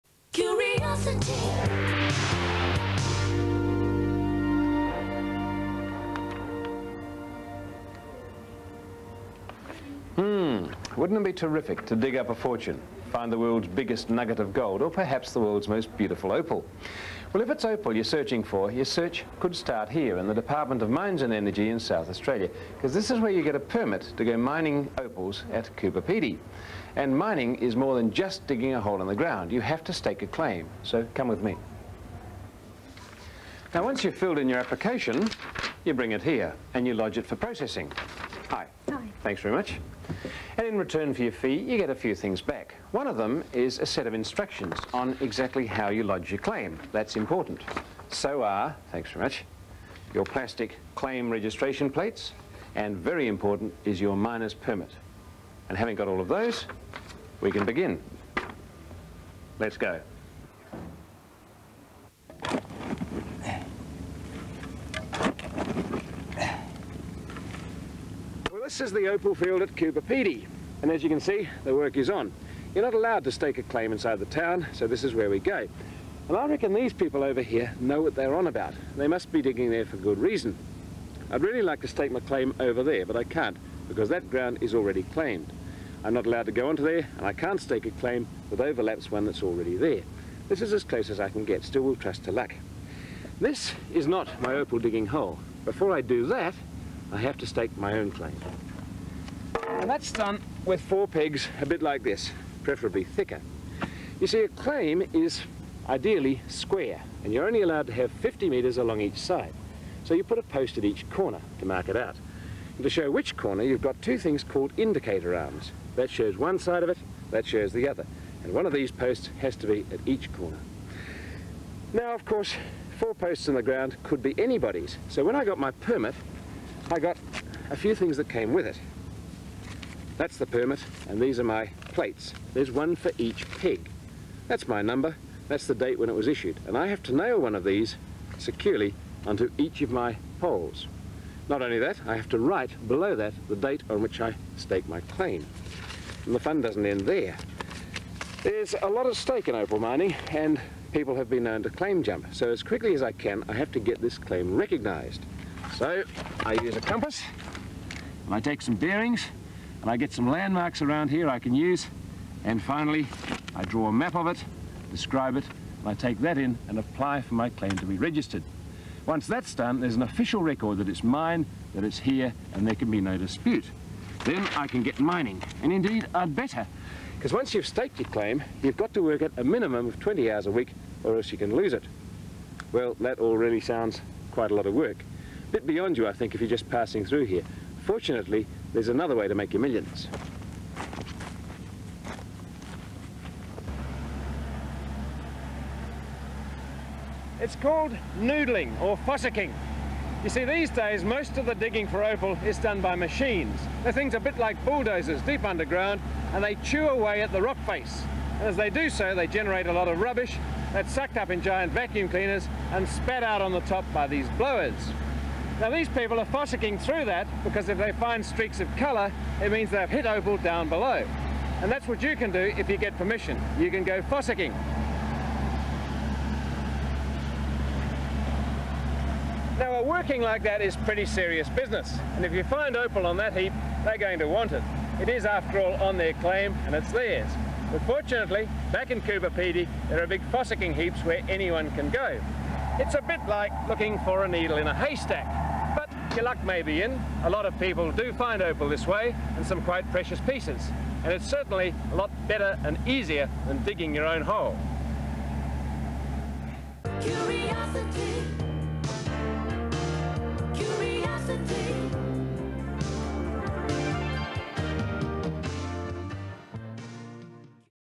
Staking a claim in an opal field
You hear of miners staking their claim, but what is involved? Rob stakes a claim in the famous South Australian opal field of Coober Pedy.